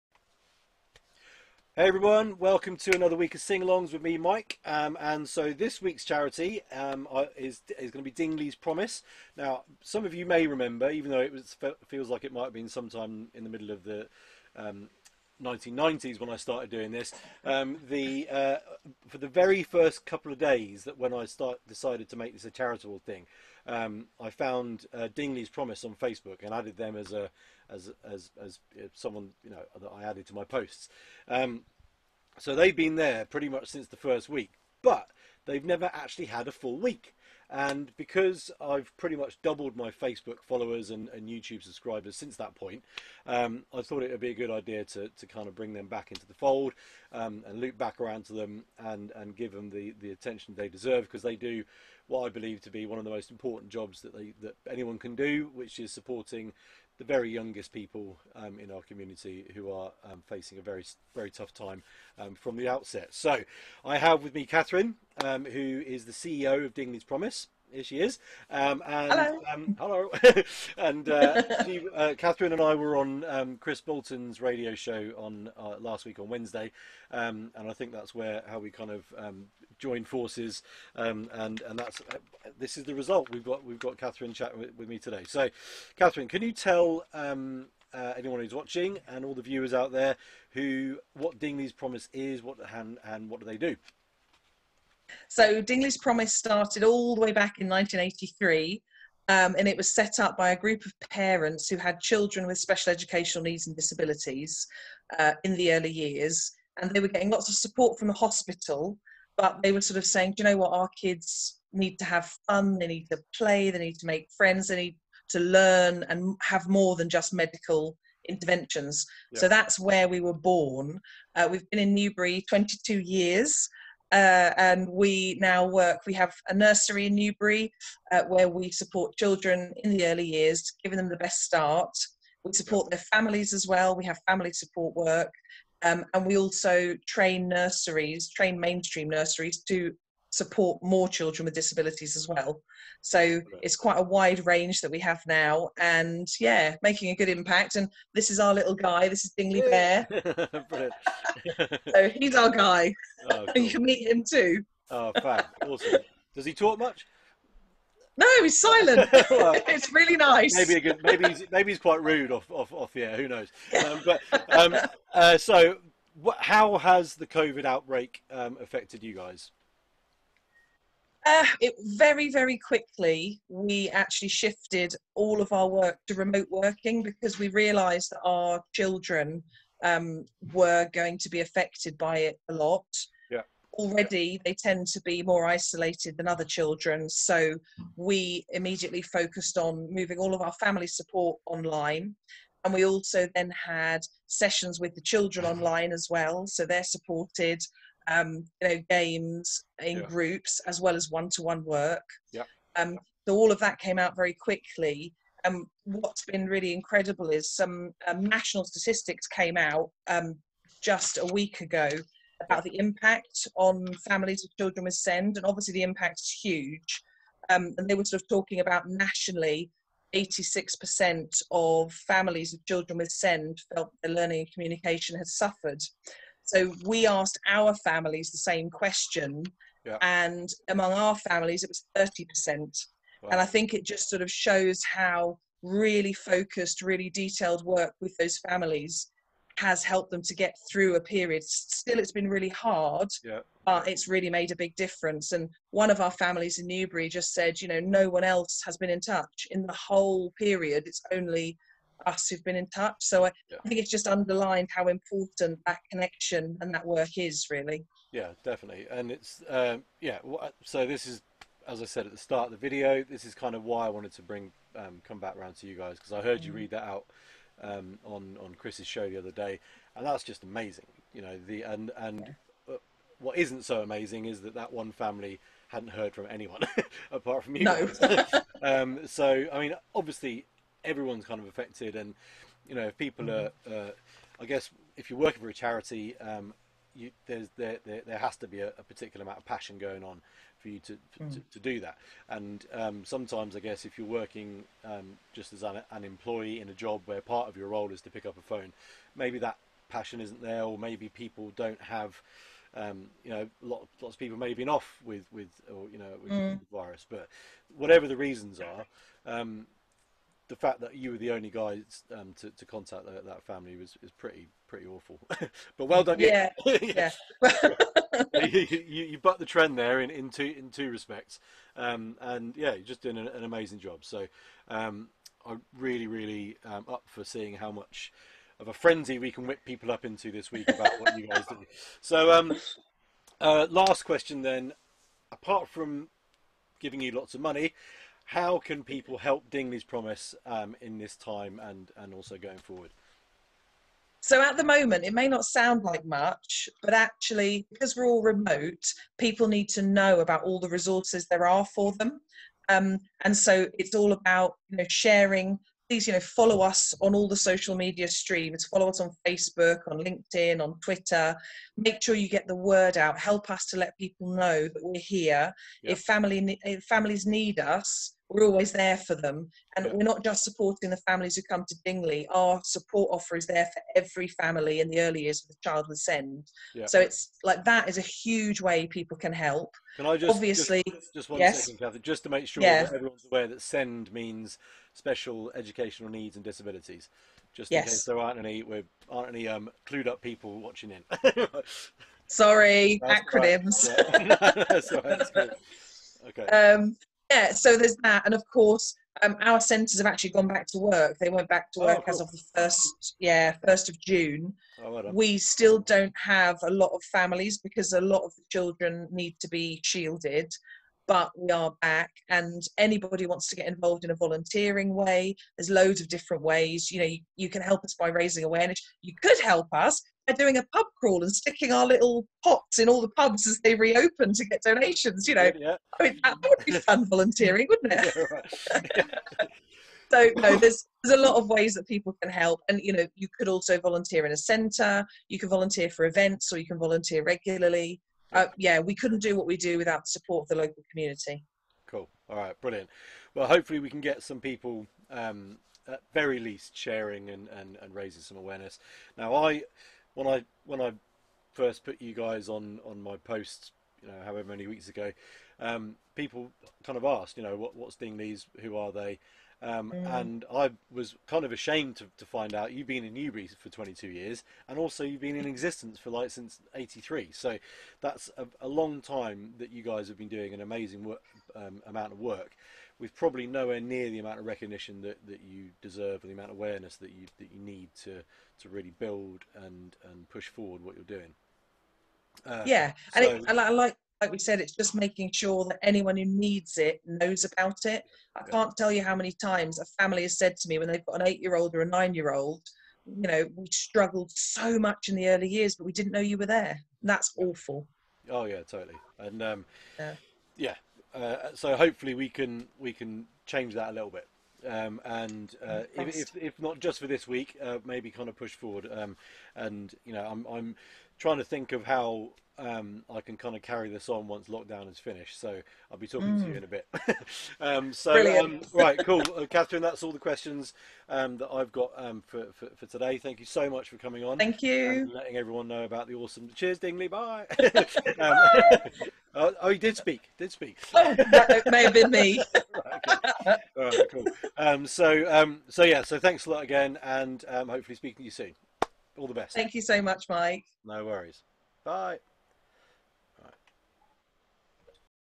Sing-along with Mike 29/06/20: Chat with Catherine from Dingley's Promise
Hi all,

This week Dingley's Promise is going to be my Charity Focus. Some of you may know that I supported Dingley's for a couple of days right at the very start of lockdown and the start of this project.

As my subscriber base has grown significantly since then (thanks everyone!!!), and as they provide such an amazing and important service, I thought it was a good time to loop around and give them a full week's worth of support.

As usual, I asked a representative from the charity to have a recorded chat with me so everyone could see what they do and how we all might be able to help them do it. And here it is!

Thanks for you time Catherine. Really great to talk with you.

Please go to their Facebook page ) to find out more!

Cheers all.

Much love,

Mike